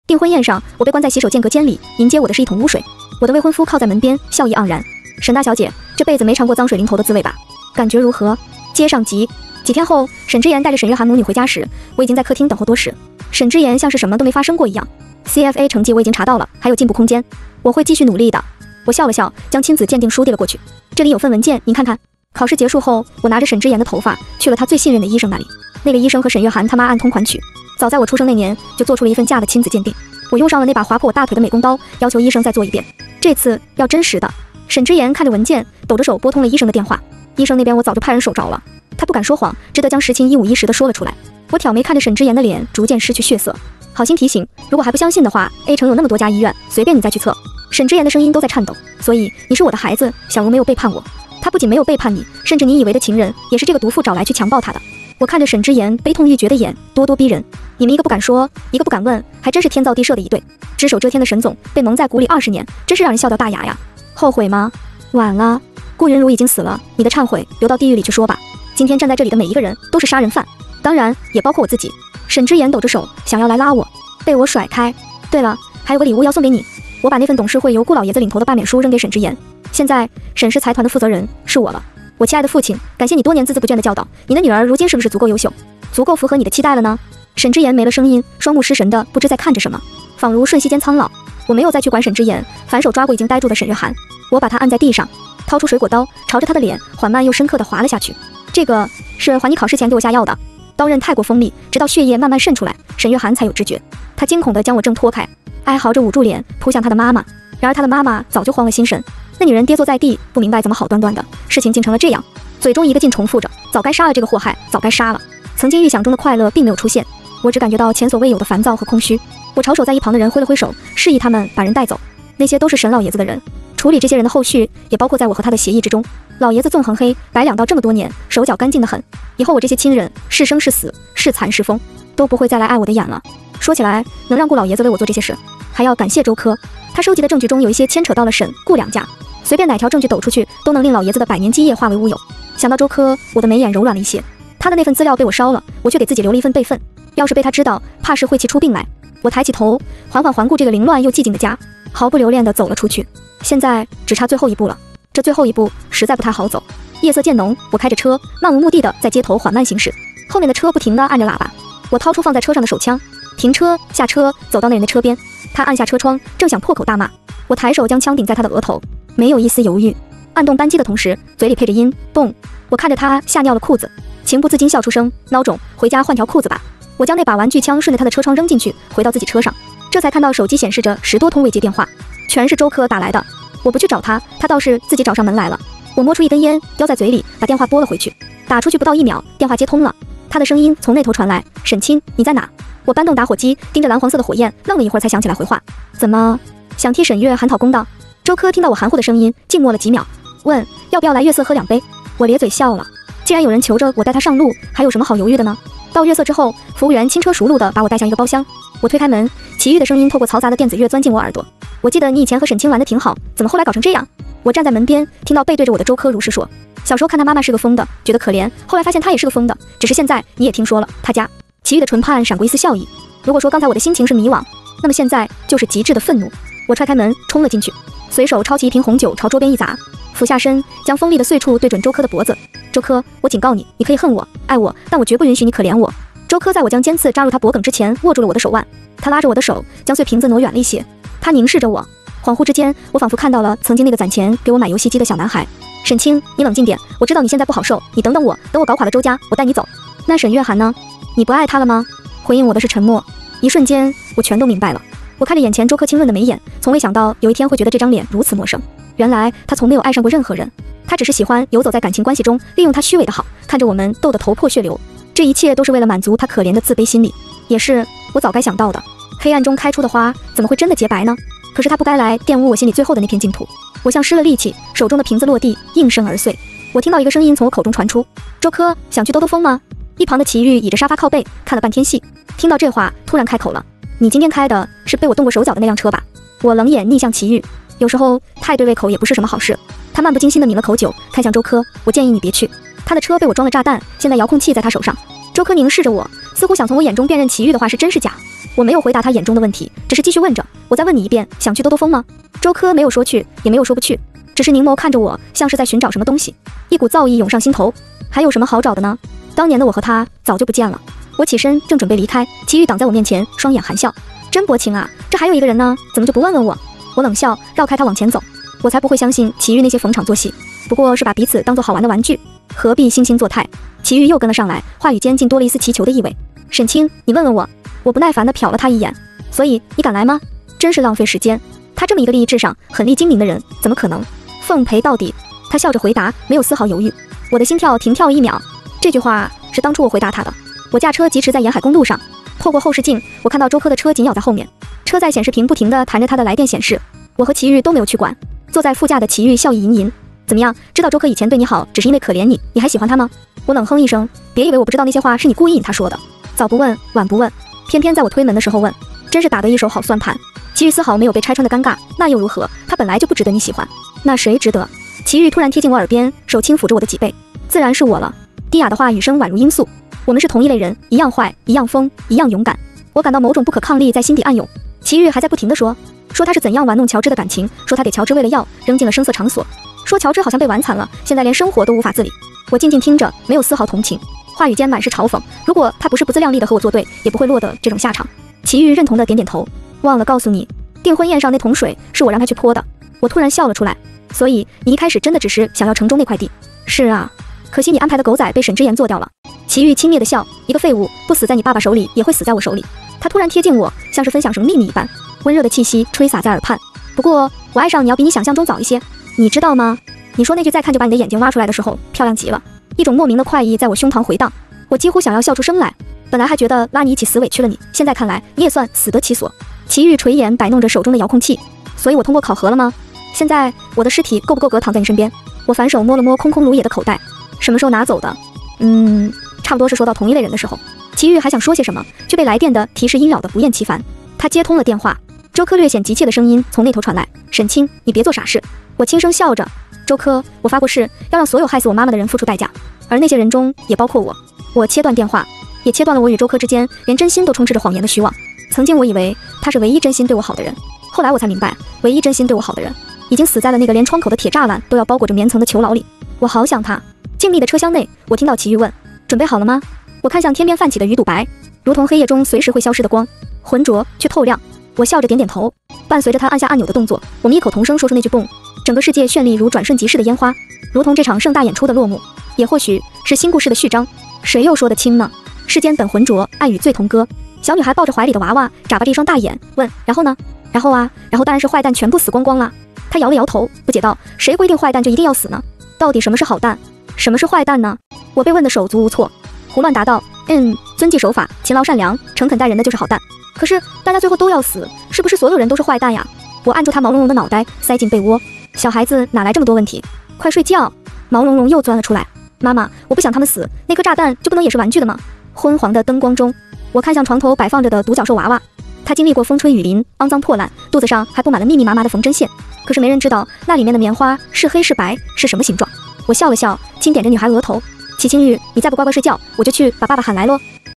《高调宠爱》已完结 | 订婚宴上，我被关在洗手间隔间里，迎接我的是一桶污水#小说
【已完结】
订婚宴上，我被关在洗手间隔间里，迎接我的是一桶污水。
我的未婚夫靠在门边，笑意盎然。
沈大小姐，这辈子没尝过脏水淋头的滋味吧？感觉如何？